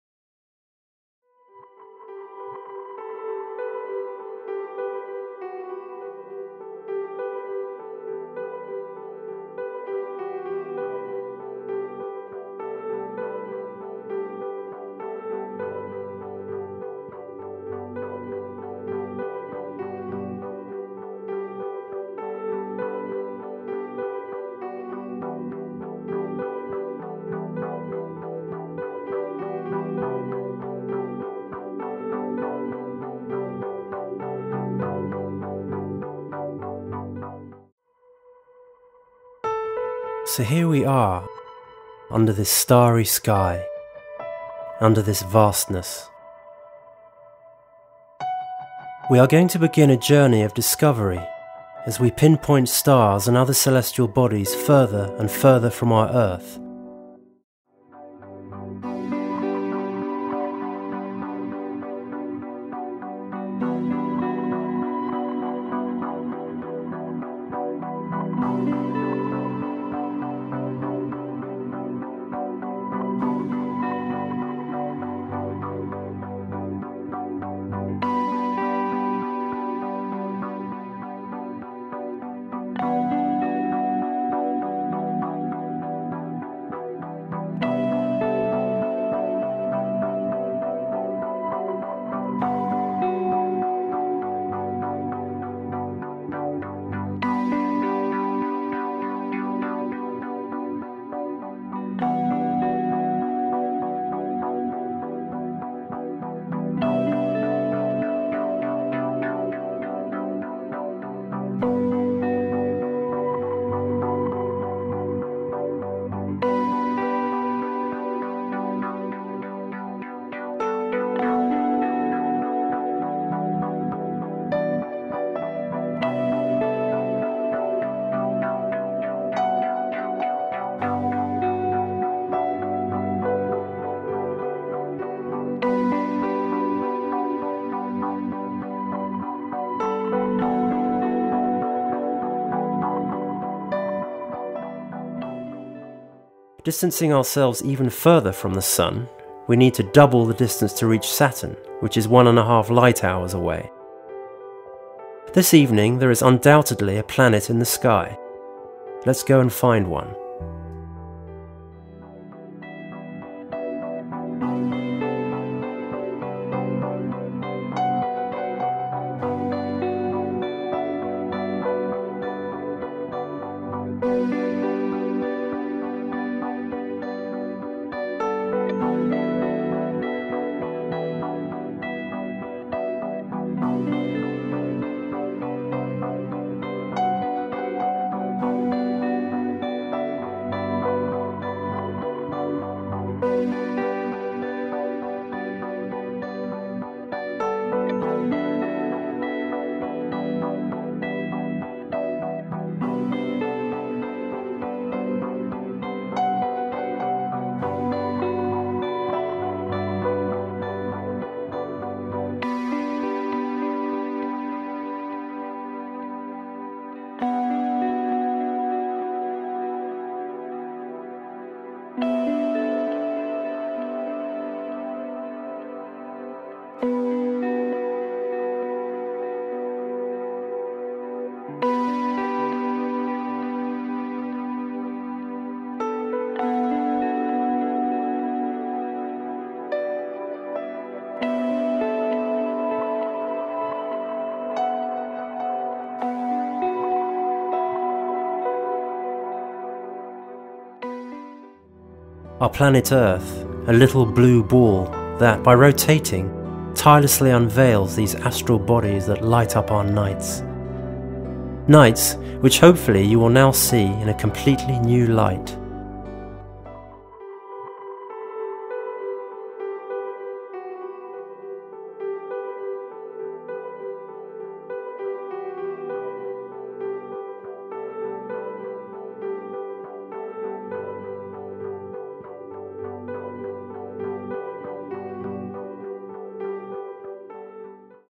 Cosmos Story user manual, iPhone, iPad and Android
The user manual of the iPhone/iPad app "Cosmos Story Vol1", the first audio guided tour of the sky.